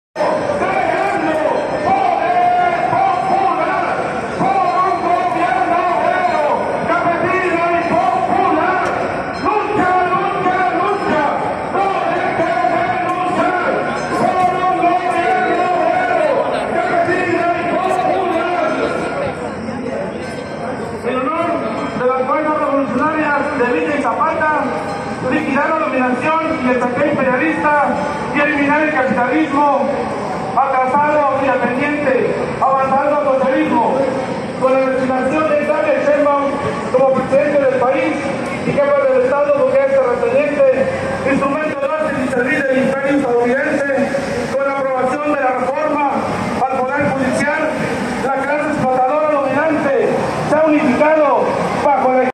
Integrantes de UPVA 28 de Octubre marcharon para conmemorar el movimiento de la Revolución Mexicana
Integrantes de la UPVA 28 de Octubre marcharon por avenida Reforma y el zócalo para conmemorar el movimiento de la Revolución Mexicana